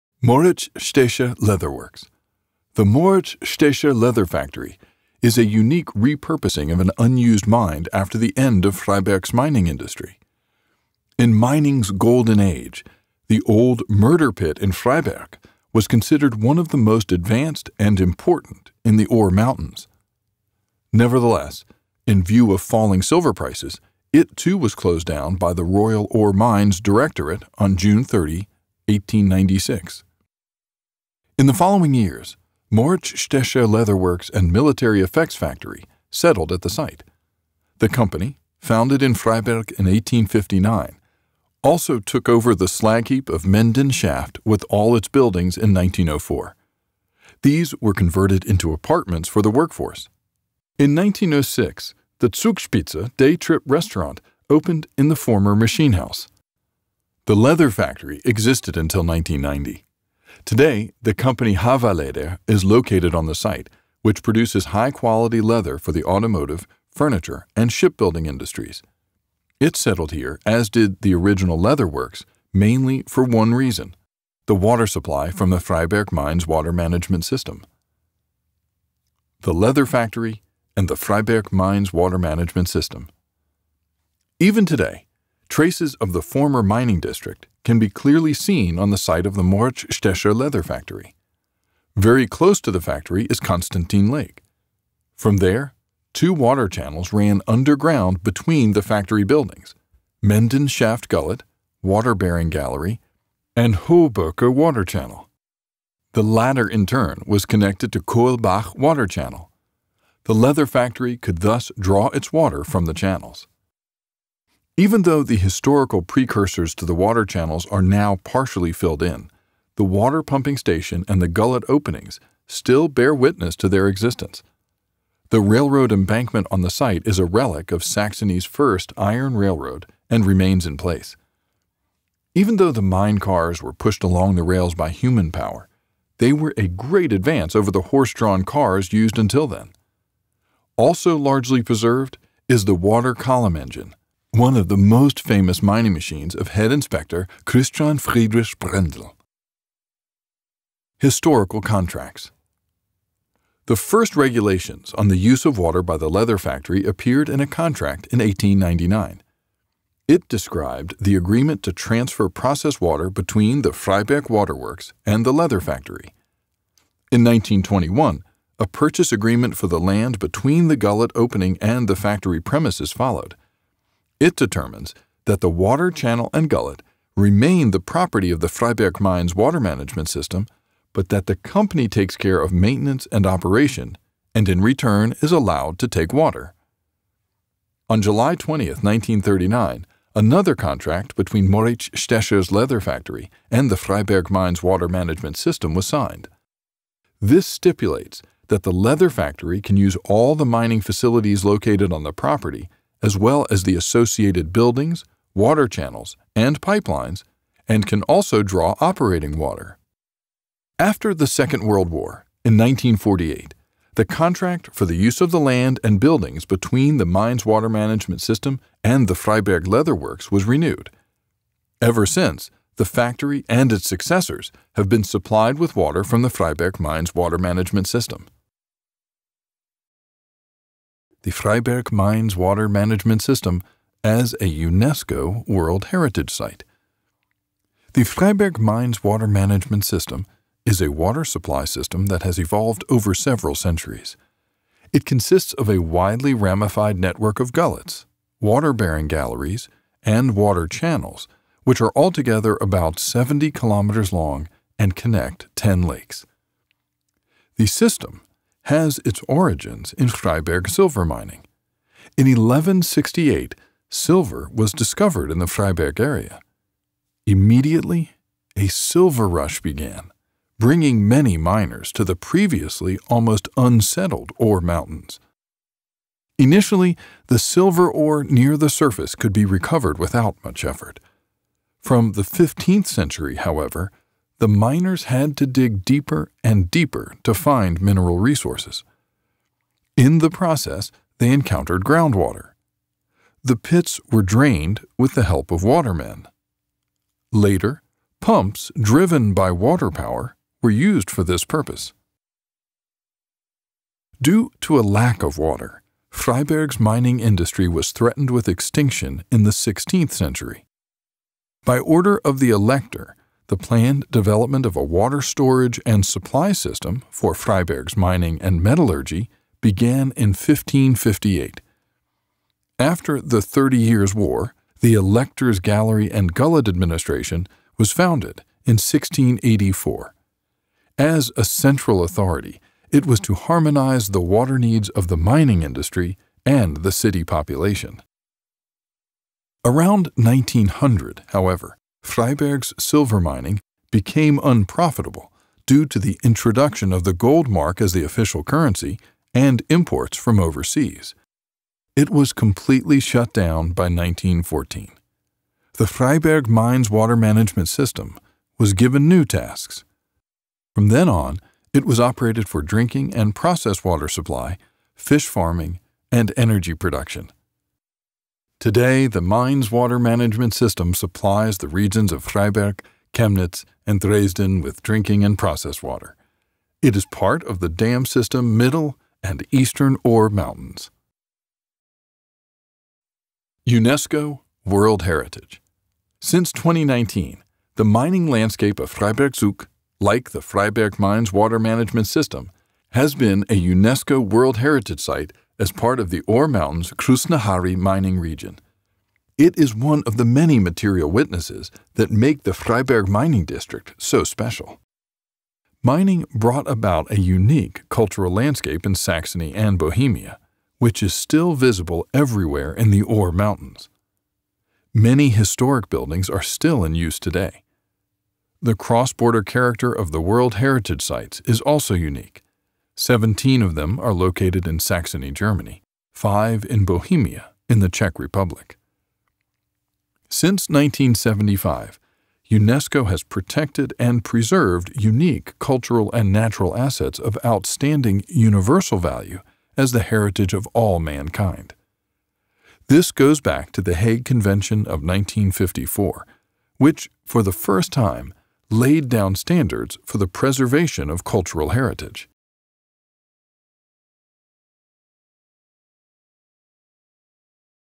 Die Lederfabrik Moritz Stecher (ENG) - Moritz Stecher Leather Works
The Moritz Stecher leather factory is a unique repurposing of an unused mine after the end of Freiberg's mining industry. The leather factory existed until 1990. Today, the company Hawa-Leder is located on the site, which produces high-quality leather for the automotive, furniture and shipbuilding industries. It settled here - as did the original leather works - mainly for one reason: the water supply from the Freiberg Mines Water Management System.

Photos: © SLUB / Deutsche Fotothek / Henrik Ahlers, Paul Schulze, Walter Möbius
Film editing: Avanga Filmproduktion